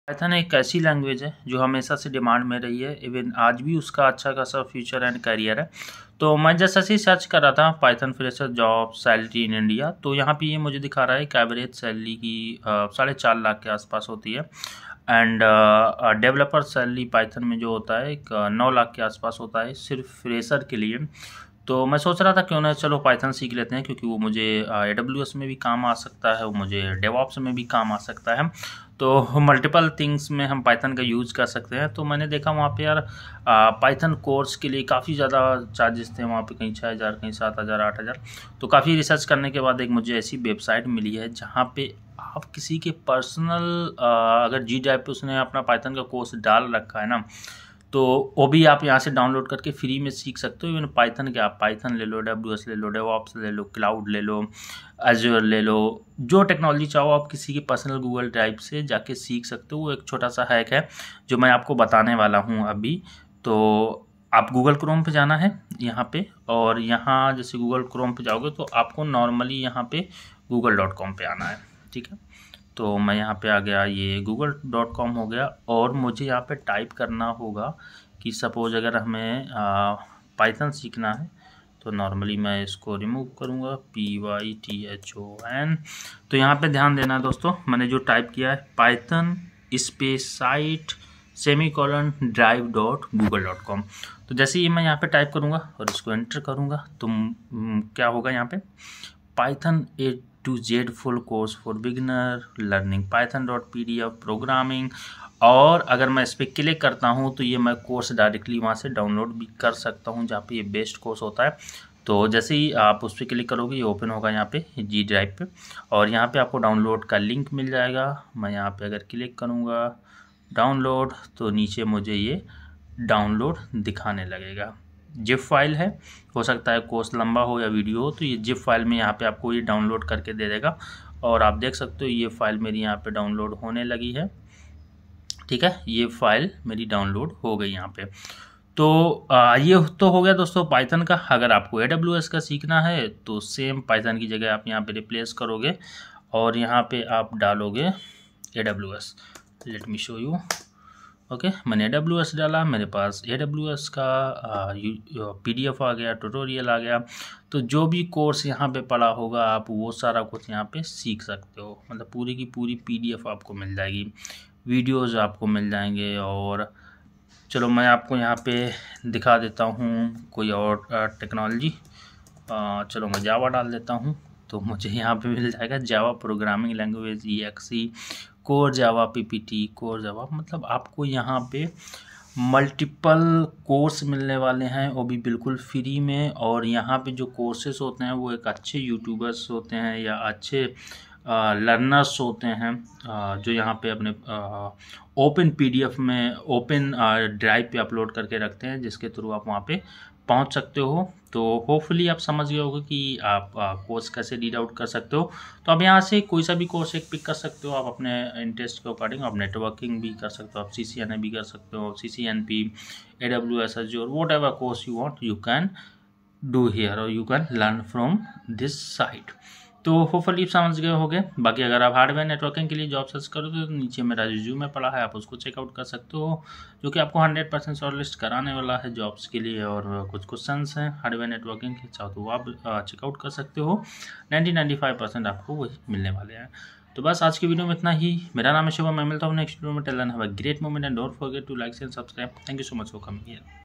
Small Hack | Python Tutorial for Beginners in Free | How to learn Programming & Python in free.Hindi
Maine is video me btaya h ki aap python and other programming course kaise free me learn kr sakte h and aap koe sa bhi course free me sikh sakte jaise ki python, aws, coding,devops, java ye sare aapko google ke drive se download kr sakte ho vo bhi free me,,i hope aapko video acha lage thanks...Small Hack | Python Tutorial for Beginners in Free | How to learn Programming & Python in free.Hindi

Thanks.